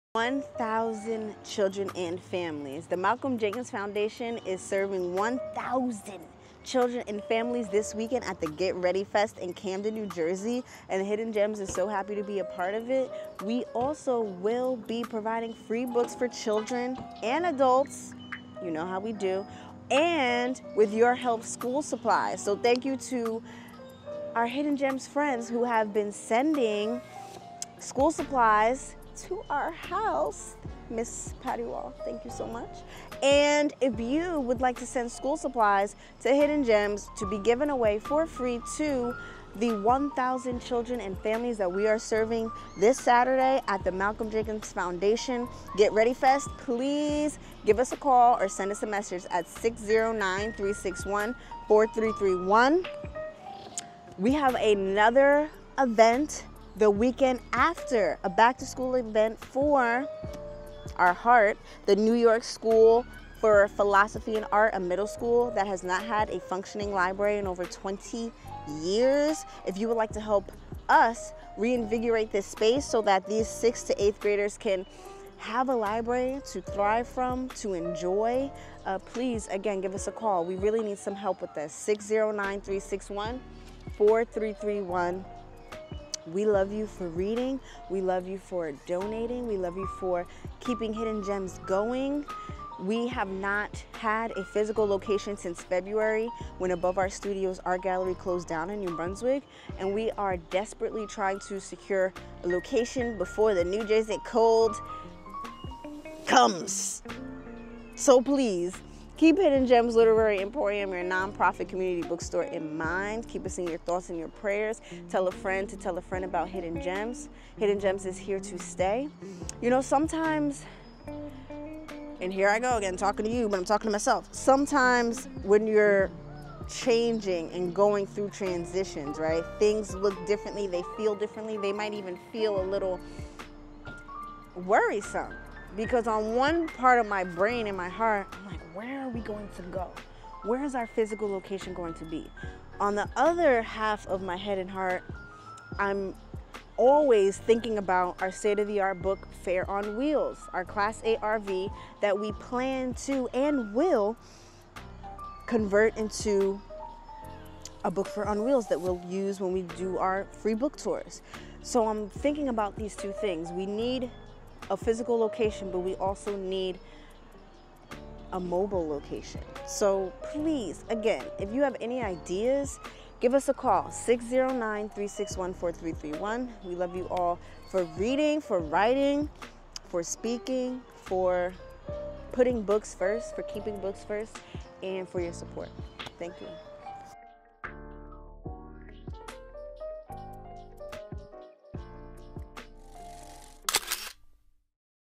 1000 Children and Families
Get ready for the Ready Fest with the Malcolm Jenkins Foundation! We will be in Camden New Jersey serving free books and school supplies to 1000 Parents and children.